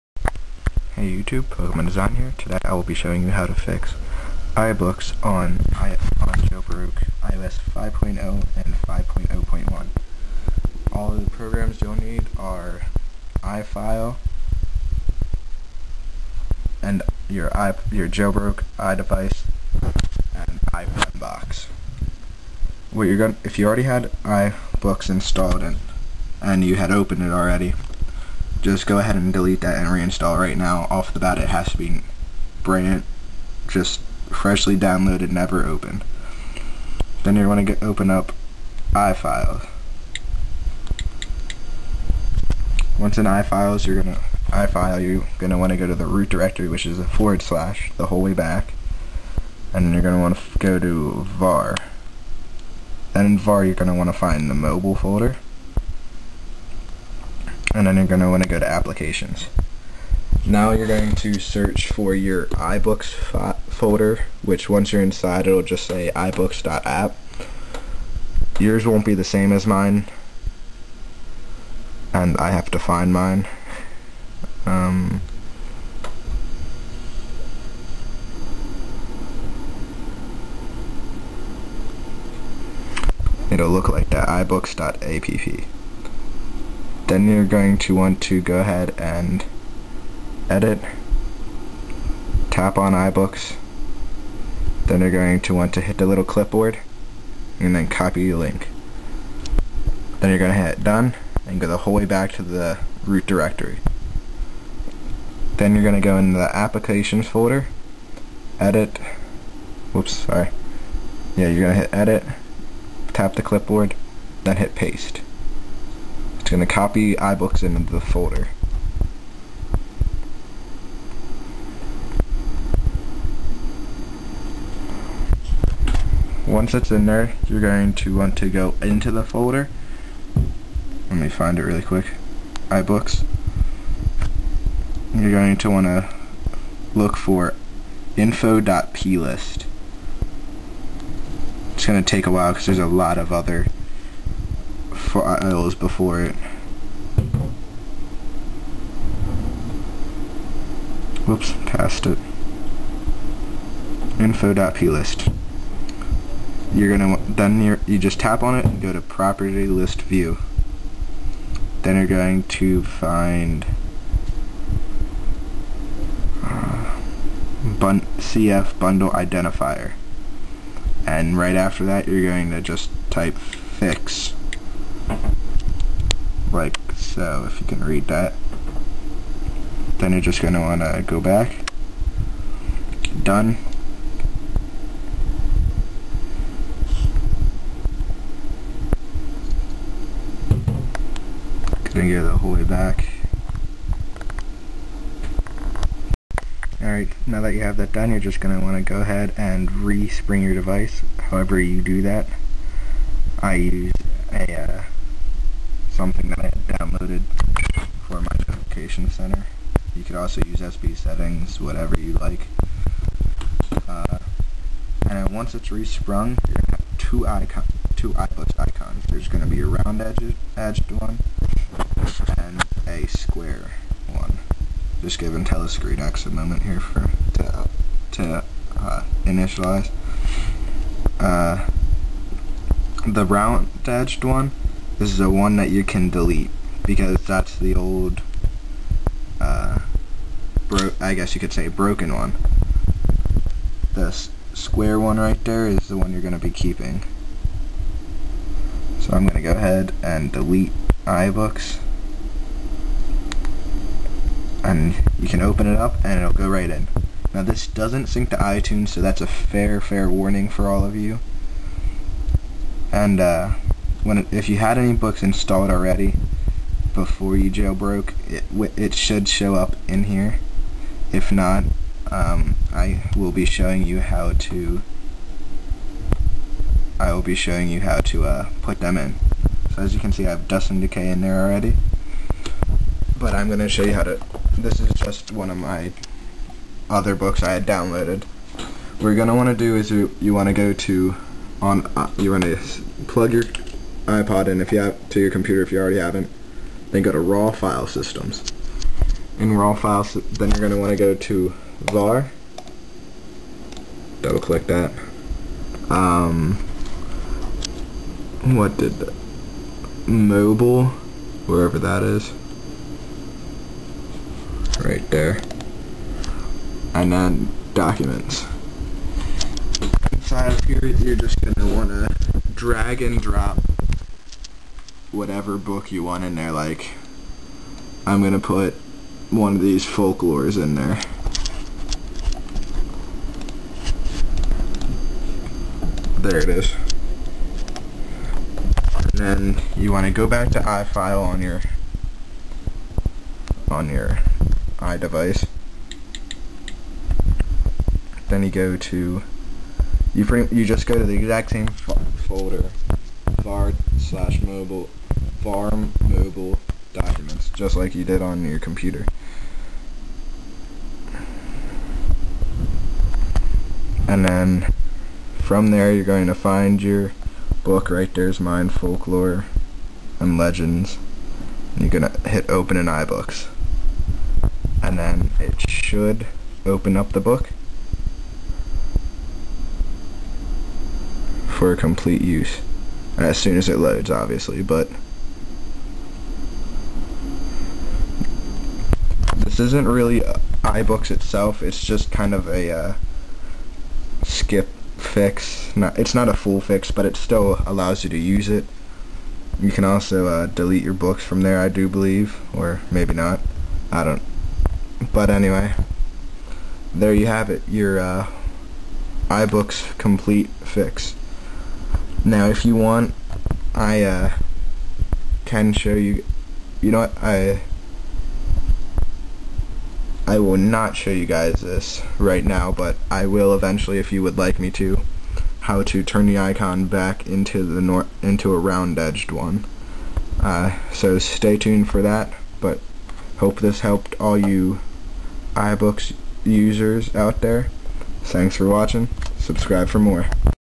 How to fix iBooks on Jailbroke iOS 5.0 and 5.0.1
This is a how to video for fixing iBooks on your jailbroke iDevice running iOS 5.0 or 5.0.1. Hope it helps.